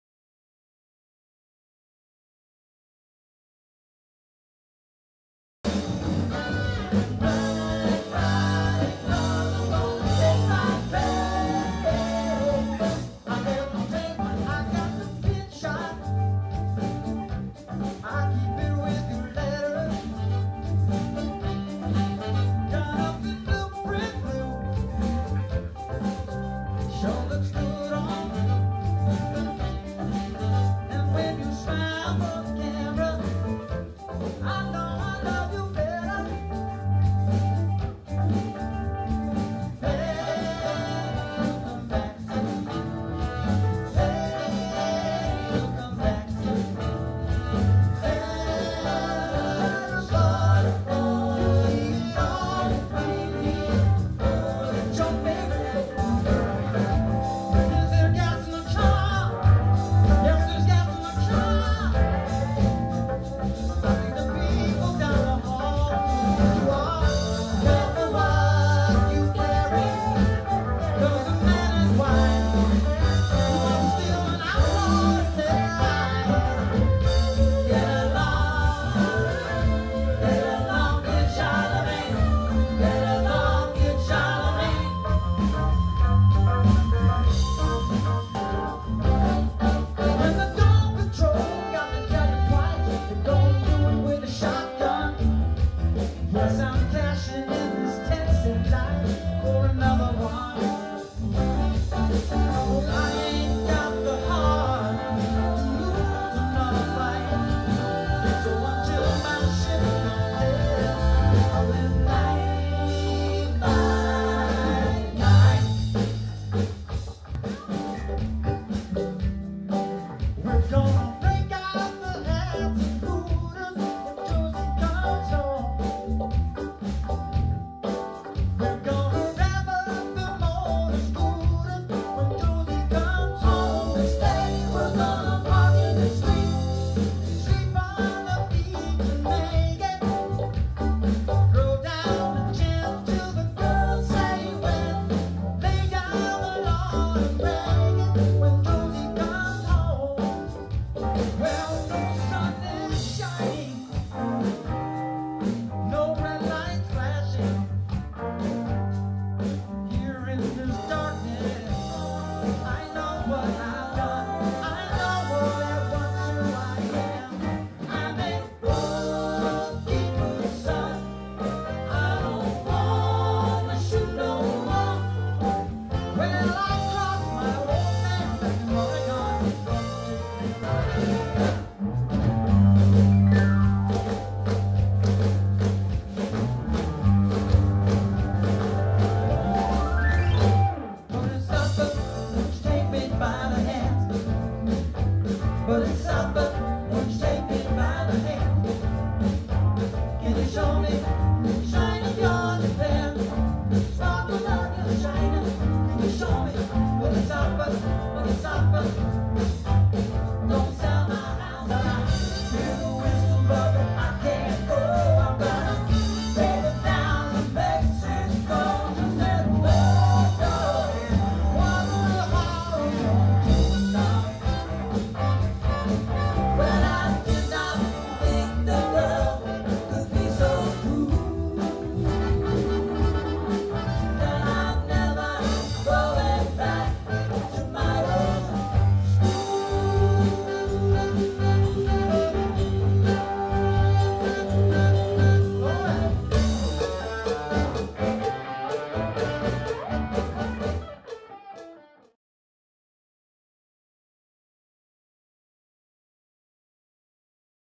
Doctor Wu Promotional Video 2013
The Doctor Wu Band
A Tribute to Steely Dan, Featuring Kenny Cetera

Kenny Cetera: Lead Vocals
Mindy Ihrke: Vocals
Leigh DeMarche: Vocals
Gil Ayan: Guitar
Steve Bias: Bass
Mark Harrison: Keyboards
Jeff Dellisanti: Saxophones
Frank Villafranca: Saxophones
Kurt Walther: Drums